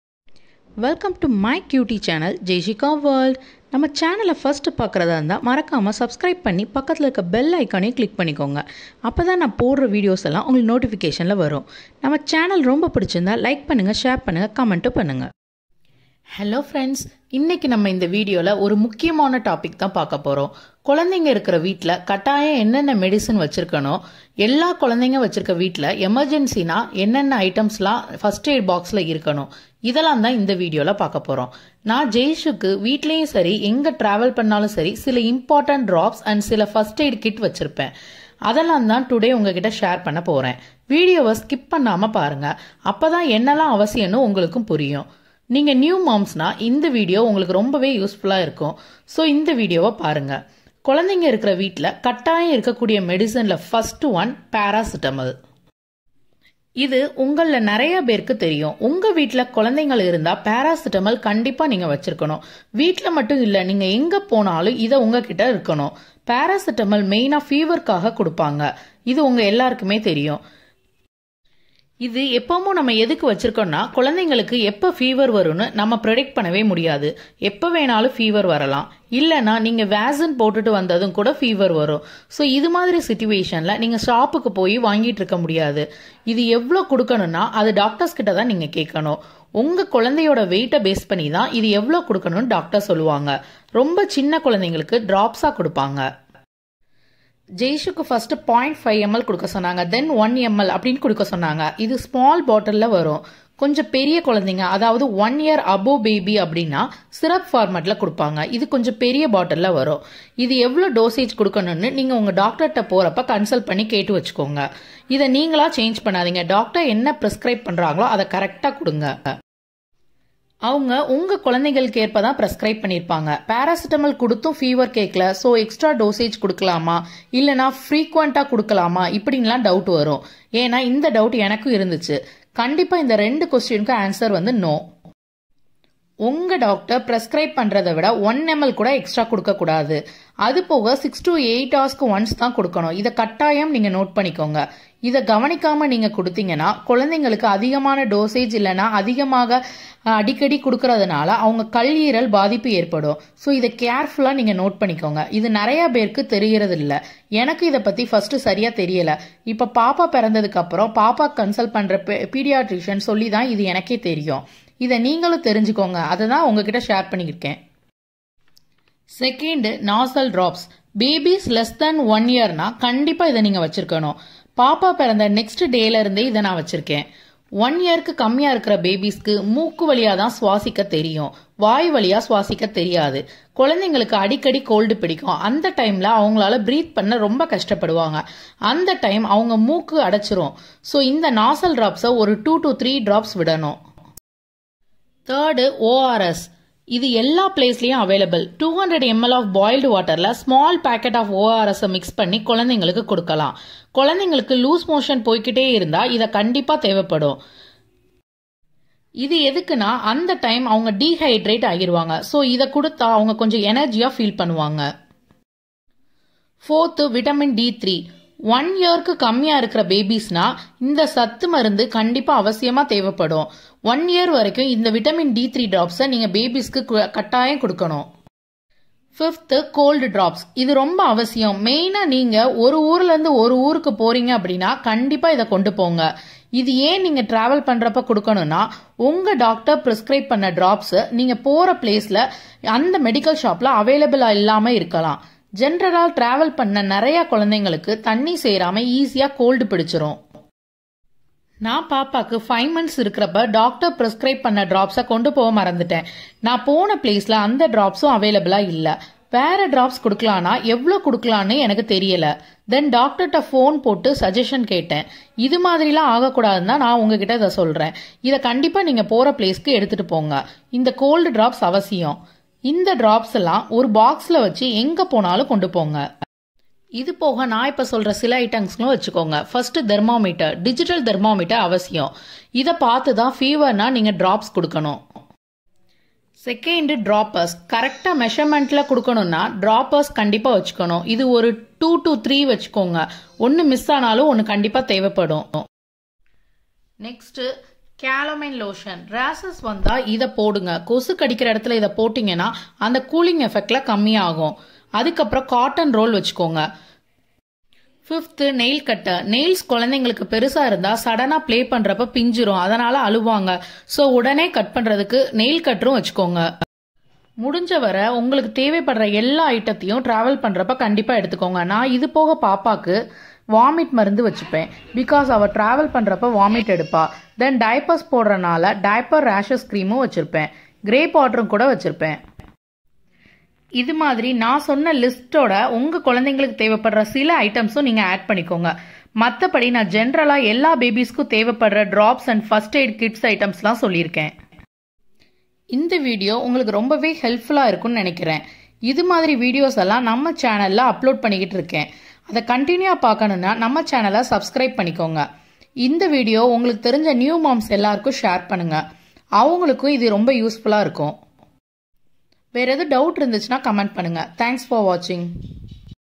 Babies first aid kit essentials in tamil||Important baby medicine list in home||Baby things to carry
Hi Pretty Pals
   This video is about baby care. If you are having babies then you have to maintain first aid kit at home and you have to carry some  essential medicines while you are traveling.I listed out several things in this video.Do watch fully it will be useful for you.